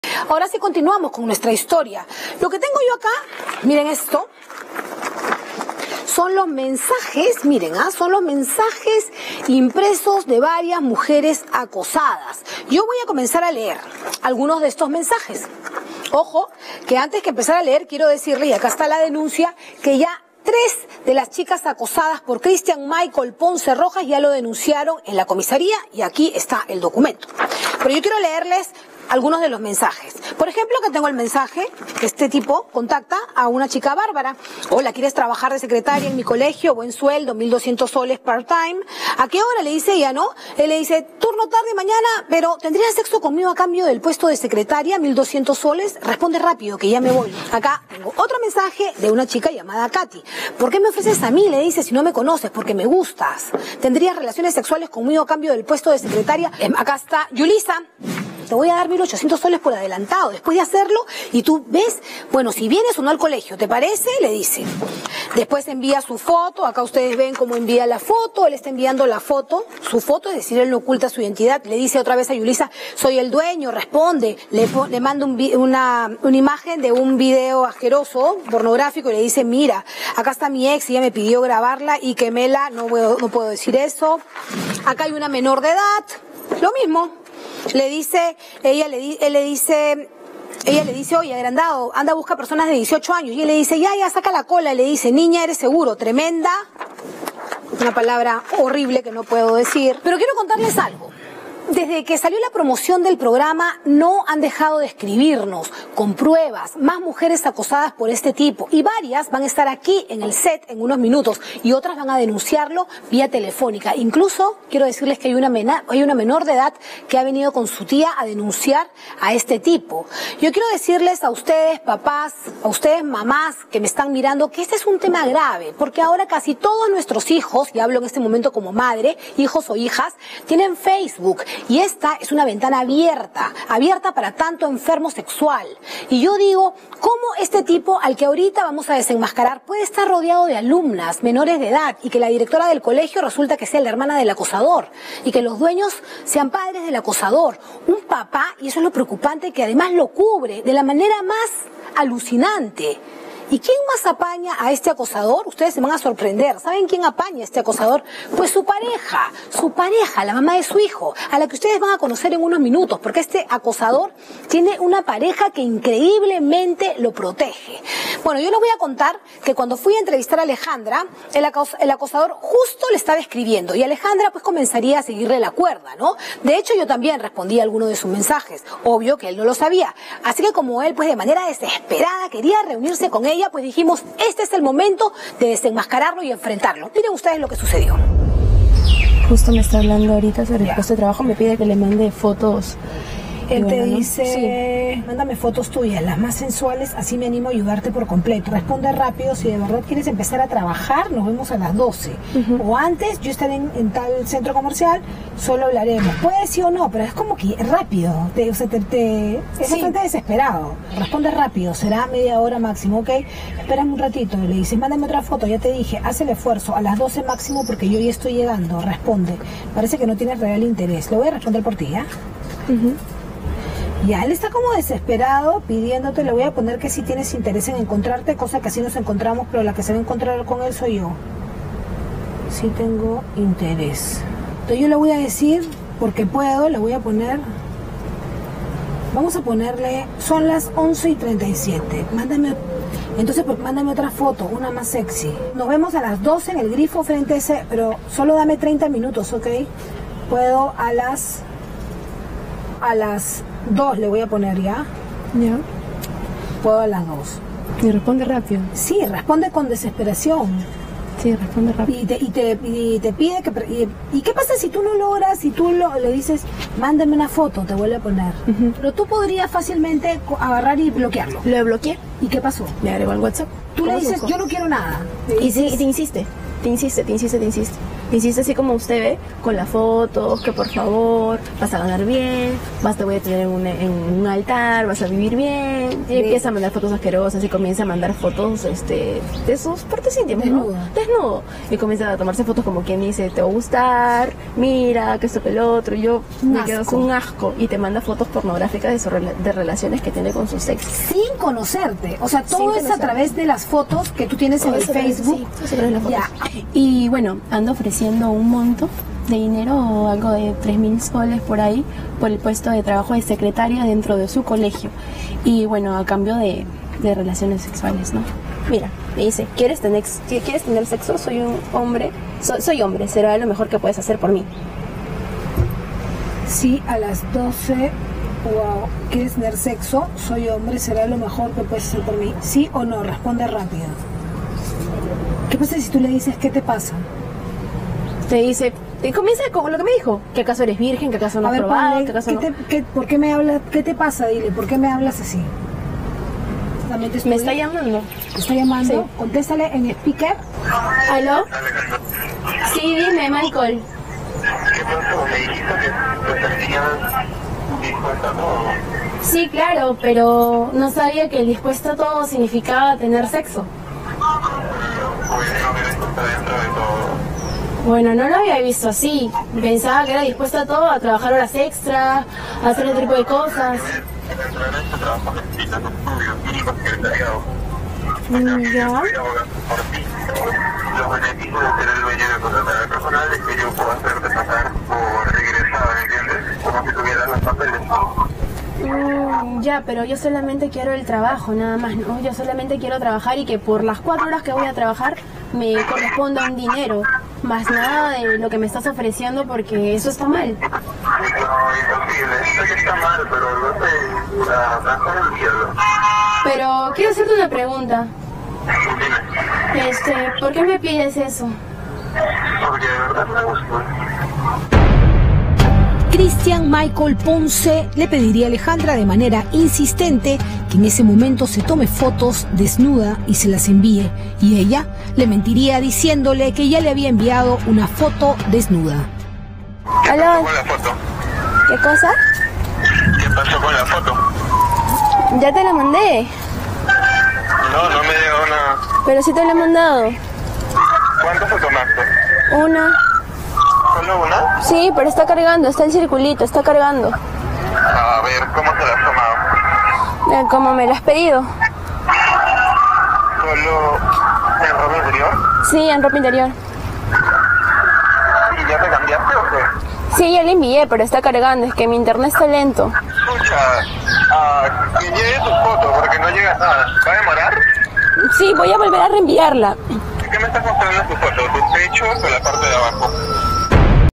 Revisa los indignantes mensajes que enviaba Cristhian Ponce a menores
La forma que tenía de operar este acosador, era aprovecharse del puesto que tenían sus padres cómo dueño de un establecimiento educacional. En dónde captaba a sus víctimas asegurando que ofrecía un puesto de secretaria, para que después de establecer el contacto comenzar a enviar mensajes en los que solicitaba sexo a las mujeres, sin importarle que muchas de ellas eran menores de edad. Incluso las hostigaba con imágenes pornográficas y vídeos en los que supuestamente tenía relaciones con una ex novia.